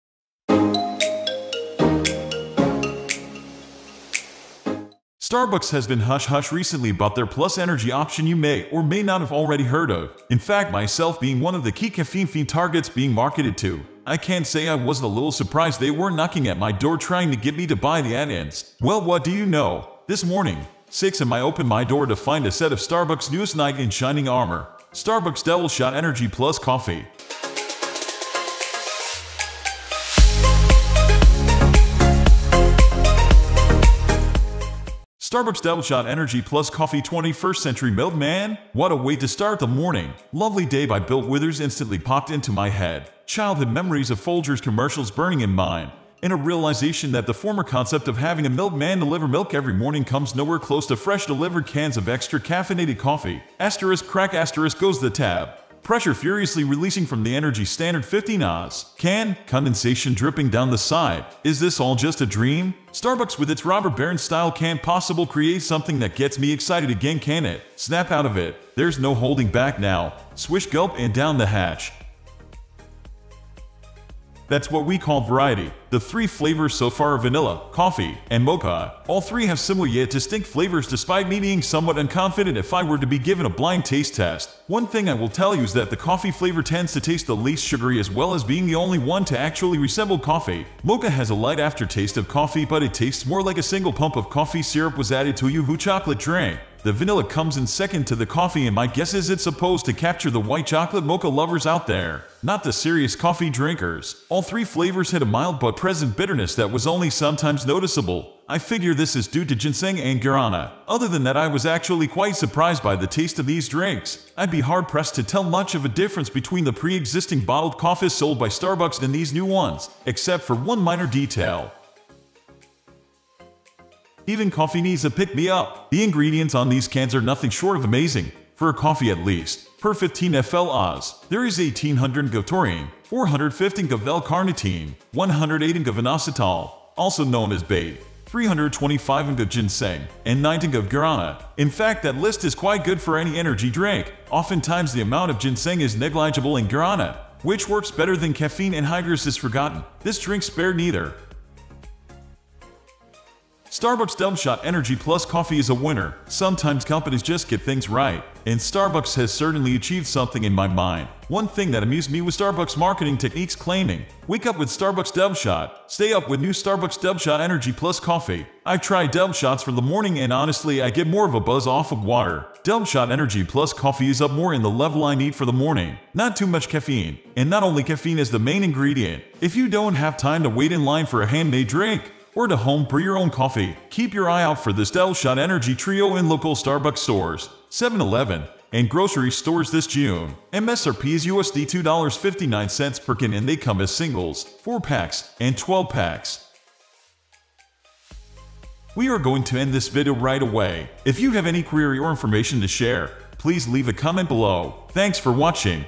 Starbucks Doubleshot Energy Coffee Beverage Review: Is It Any Good?!
Starbucks has been hush-hush recently about their +Energy option you may or may not have already heard of.

In fact myself being one of the key caffeine fiend targets being marketed to, I can’t say I wasn’t a little surprised they weren’t knocking at my door trying to get me to buy the add-ins. Well what do you know, this morning (6am) I open my door to find a set of Starbucks’ newest knight in shining armor, Starbucks DoubleShot Energy + Coffee.